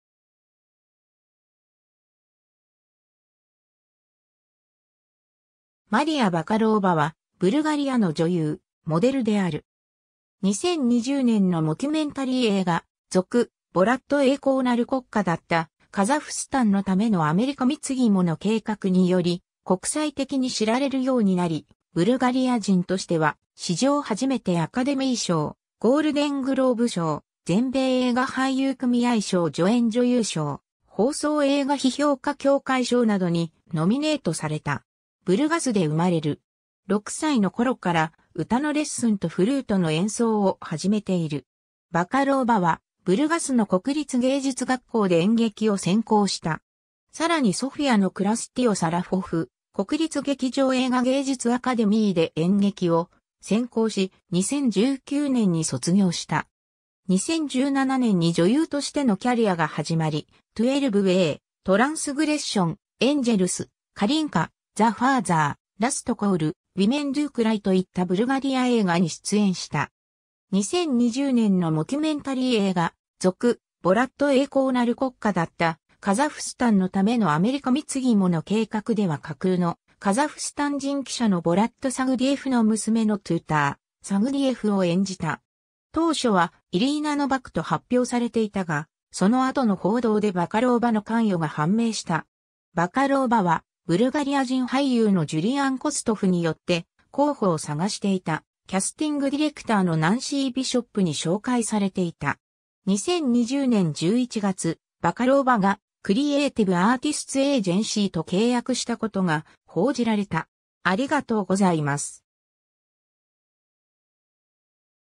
マリア・バカローヴァ
マリア・バカローヴァ（ブルガリア語: Мария Бакалова [mɐˈrijɐ bɐˈkaɫovɐ]; 1996年6月4日 - ）は、ブルガリアの女優、モデルである。
2020年のモキュメンタリー映画『続・ボラット 栄光ナル国家だったカザフスタンのためのアメリカ貢ぎ物計画』により国際的に知られるようになり、ブルガリア人としては史上初めてアカデミー賞、ゴールデングローブ賞、全米映画俳優組合賞助演女優賞、放送映画批評家協会賞などにノミネートされた。
 ブルガスで生まれる。
6歳の頃から歌のレッスンとフルートの演奏を始めている。
バカローヴァはブルガスの国立芸術学校で演劇を専攻した。
さらにソフィアのクラスティオ・サラフォフ国立劇場映画芸術アカデミーで演劇を専攻し、2019年に卒業した。
 2017年に女優としてのキャリアが始まり、『XII A』（2017年）、『Transgression』（2018年）、『Angels』（2018年）、『Kalinka』（2018年）、『The Father』（2019年）、『Last Call』（2020年）、『Women do cry』（2021年）といったブルガリア映画に出演した。
 2020年のモキュメンタリー映画『続・ボラット 栄光ナル国家だったカザフスタンのためのアメリカ貢ぎ物計画』では架空のカザフスタン人記者のボラット・サグディエフの娘のトゥーター・サグディエフを演じた。
当初はイリーナ・ノヴァクと発表されていたが、その後の報道でバカローヴァの関与が判明した。
バカローヴァはブルガリア人俳優のジュリアン・コストフによって候補を探していたキャスティングディレクターのナンシー・ビショップに紹介されていた。
 2020年11月、バカローヴァがクリエイティヴ・アーティスツ・エージェンシーと契約したことが報じられた。